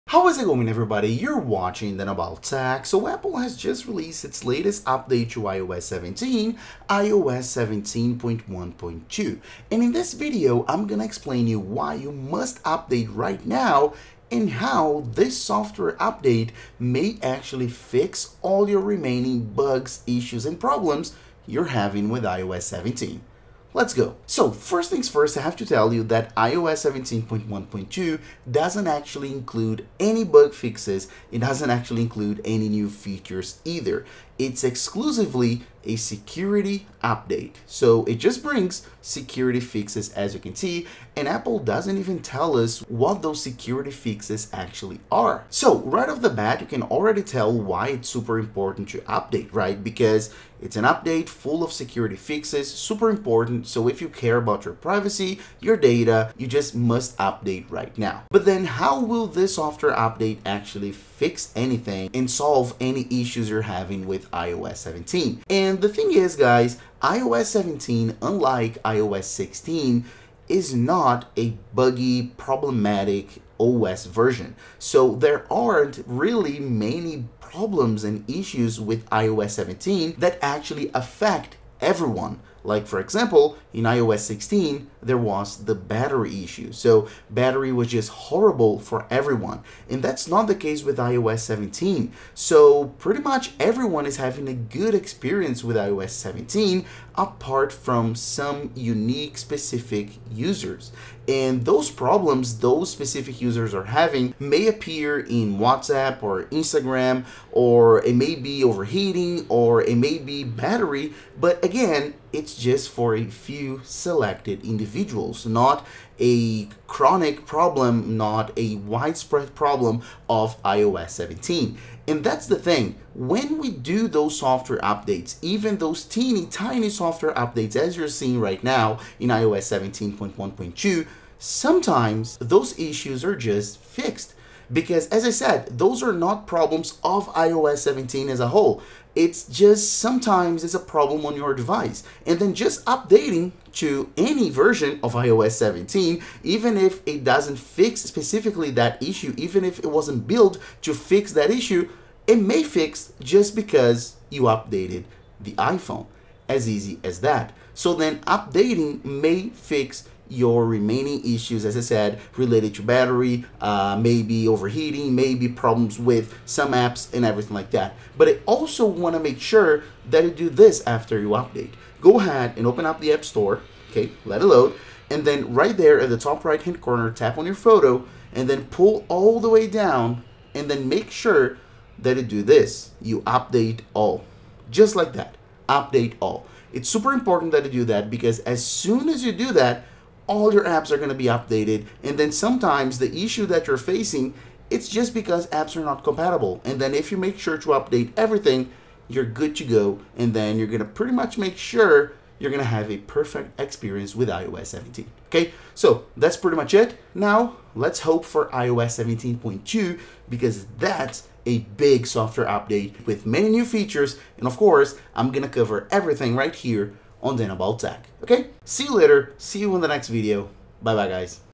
iOS 17.1.2 - This Software Update May Fix All Your Bugs and Issues!
Today I talk about iOS 17.1.2, a super important security software update for everyone and that may fix all your issues, problems and bugs! Hope U Enjoy!